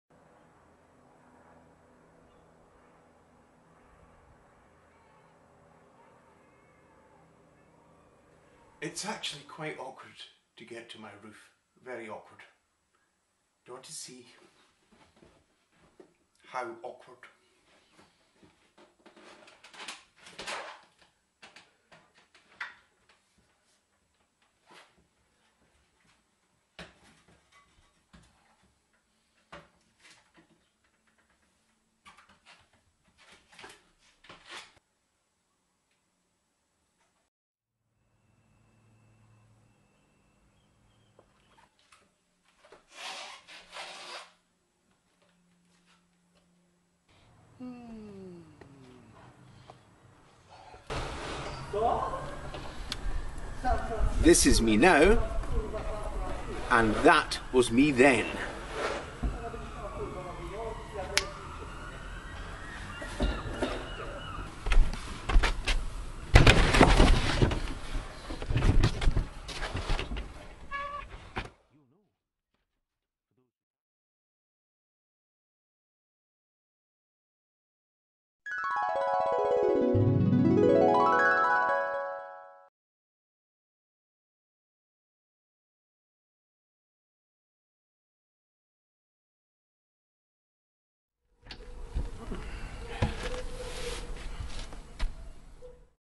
Ray's On That Roof Again
It's not exactly the world's most accessible or the world's safest sunroof, especially when your chair falls apart. Enjoy many more fun videos like this on the playlists below.